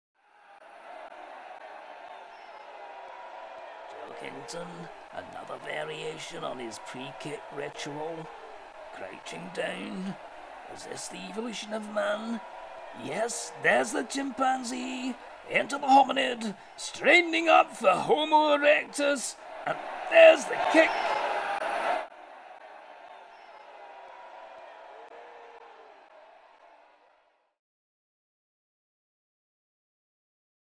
Rugby Kick Chic - Evolution
The rugby place-kick has come a long way since Johnny Wilkinson brought us the 'little bird in the hand'. And he's a hard act to follow. Rugby's a brutal game... in which you're judged by your last kick.

Also see:
Rugby Kick Chic - Flight Attendant; and
Rugby Kick Chic - Crossword